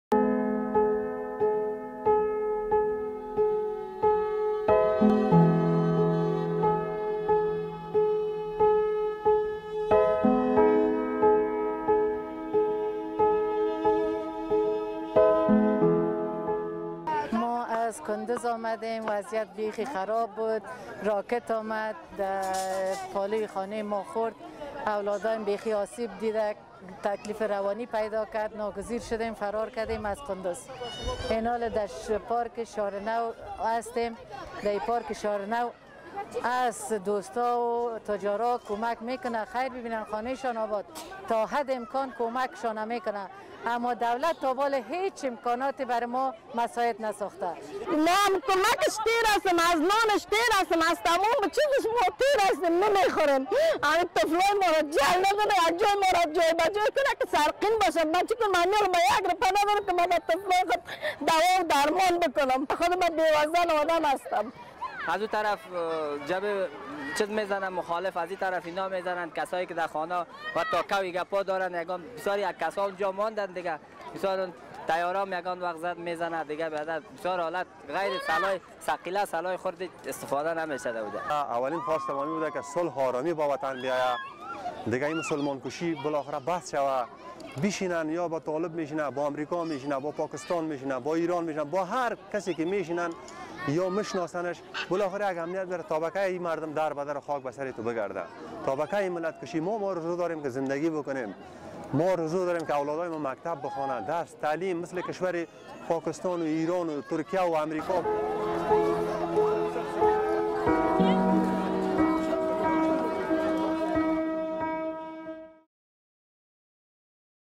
بیجاشدگان جنگ‌ نیاز فوری به کمک دارند
در نتیجهٔ حملات طالبان در شماری از ولایت‌های افغانستان، شمار زیادی از مردم مجبور به ترک مناطق اصلی شان شده و به شهر کابل پناه آوردده اند. شماری از این بیجاشدگان که در پارک شهر نو کابل در فضای باز به سر می‌برند، نیاز فوری به آب، غذا، سرپناه و تامین ضرورت های ابتدایی برای زنده ماندن دارند.